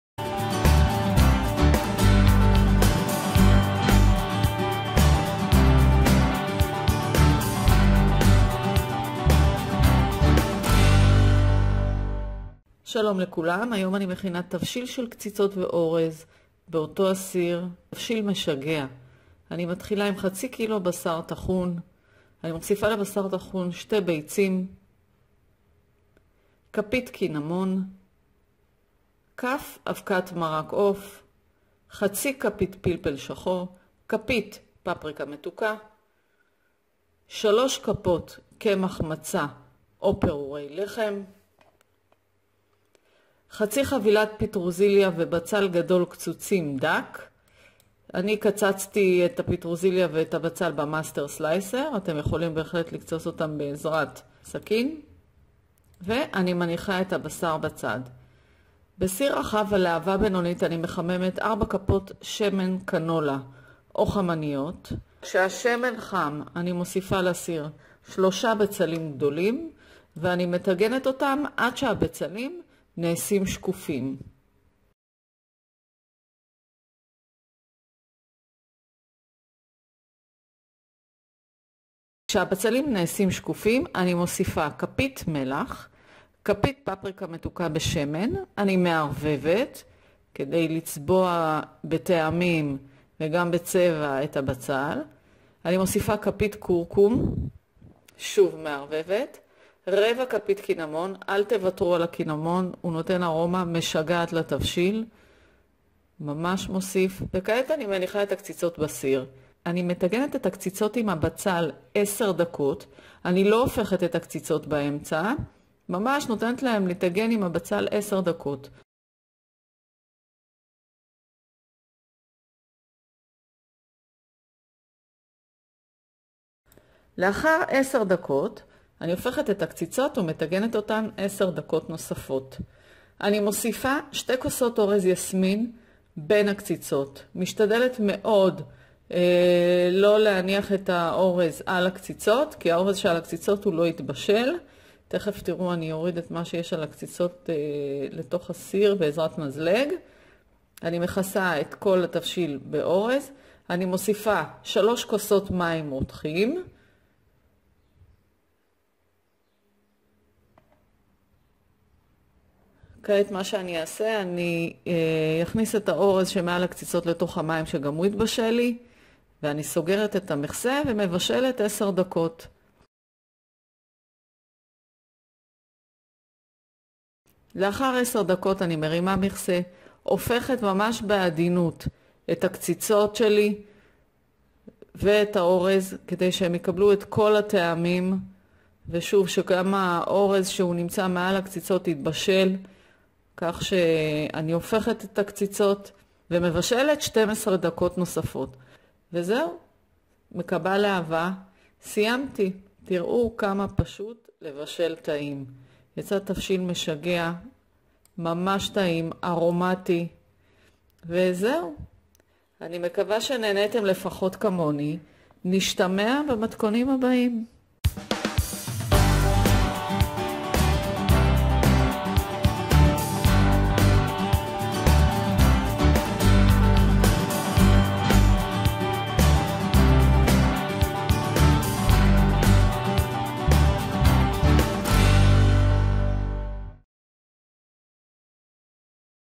תבשיל קציצות בשר ואורז באותו הסיר
תבשיל קציצות בשר ואורז באותו הסיר. המתכון המלא בעברית כאן
A bowl of patties and rice in the same pot
Ingredients
2 cups jasmine rice
3 cups boiling water
3 large onions
1 teaspoon sweet paprika in oil
1 teaspoon turmeric
1/4 teaspoon ground cinnamon
1 teaspoon salt
4 tablespoons sunflower oil or canola
Ingredients for meatballs
A pound of neck meat or fresh ground chops
Leaves from half a package of parsley
3 tablespoons matzo flour or breadcrumbs
1 medium onion
2 eggs
Half a teaspoon of black pepper
Spoon of sweet paprika
Tablespoon chicken soup powder
1 teaspoon ground cinnamon
Instructions
Finely chop the slicer master leaves from half a package of parsley, 1 onion and transfer to a bowl. Add 1 pound of minced meat, 2 eggs, 1 tablespoon chicken soup mix, 1 teaspoon sweet paprika, half teaspoon black pepper, 3 tablespoons matzo flour, mix well and set aside.
Slice 3 large onions into rings and set aside. Heat in a wide saucepan, medium flame 4 tablespoons sunflower oil or canola. When the oil is hot add rings from three onions and fry until the onions become transparent. Add a teaspoon of sweet paprika, a teaspoon of turmeric, a teaspoon of salt, a quarter teaspoon of cinnamon and fry for about 2 minutes.
After 2 minutes, create meatballs and sprinkle them with a pot. Add 2 cups of jasmine rice between the patties and around them, pour 3 cups boiling water, close the lid and cook for 10 minutes. After 10 minutes turn the patties and rice to make the rice cook from all directions, close the lid and cook for another 12 minutes until the rice is ripe.